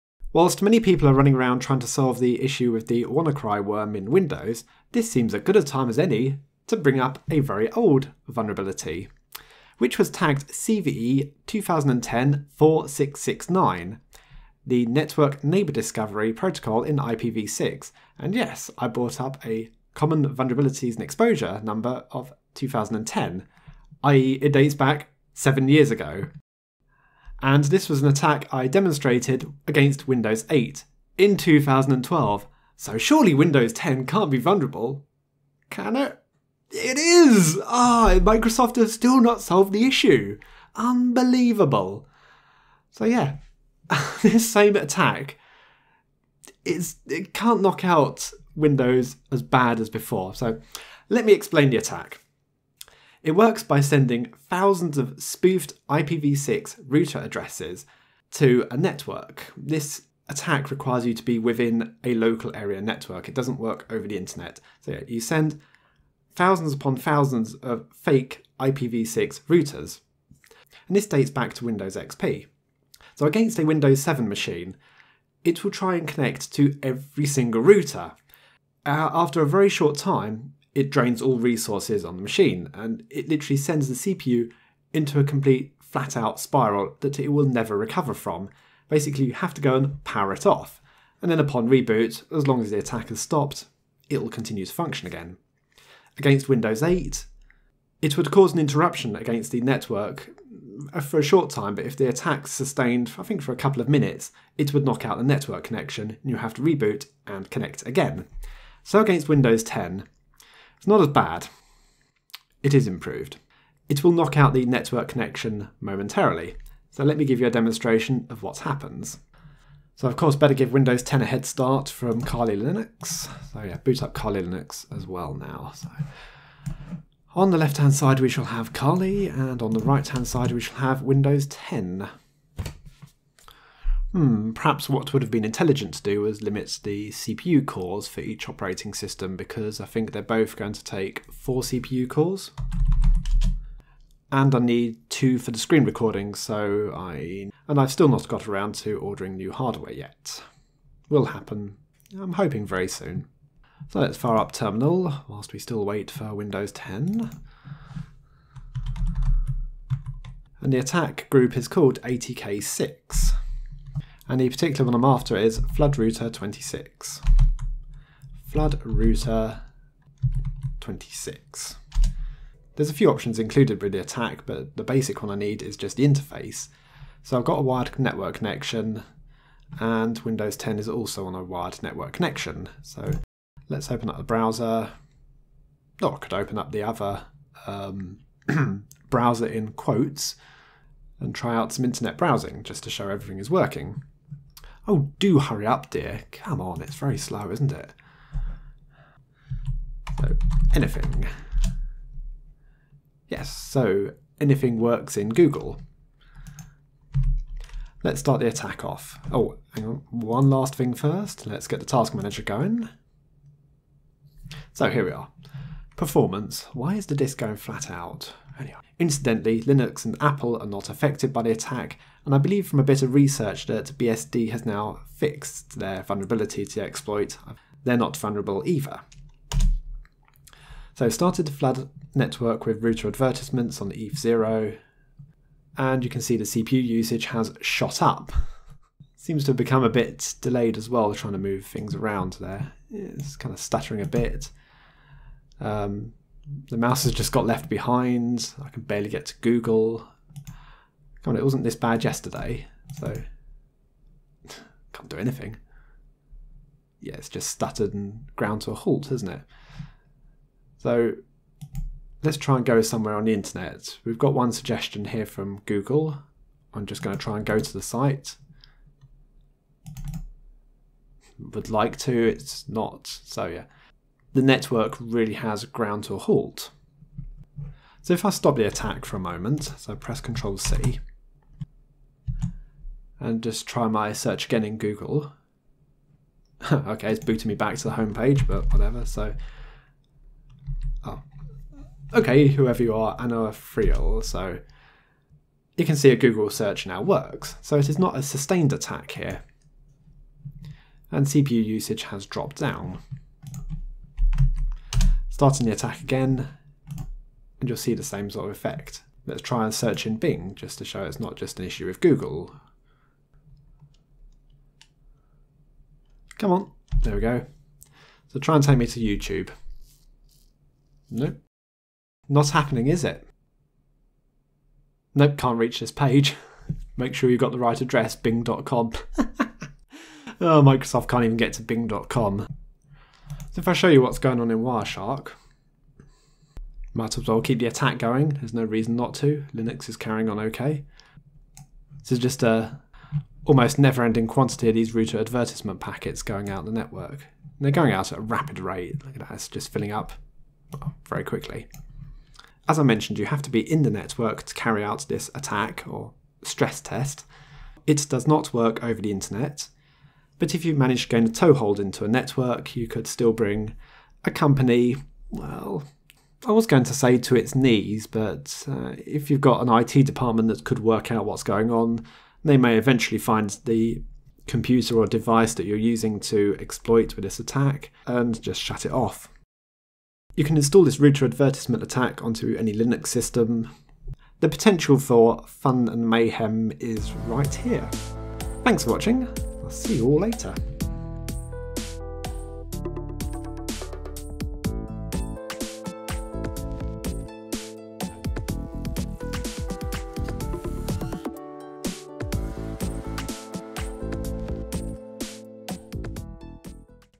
DoS Attack Windows 10 With Old 2010 Vulnerability
Causing a Denial of Service attack against Windows 10 with an old IPv6 Router Advertisement Attack (CVE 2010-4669). The same attack I demonstrated against Windows 8 back in 2012.

Kali Linux attack command:
# atk6-flood_router26 eth0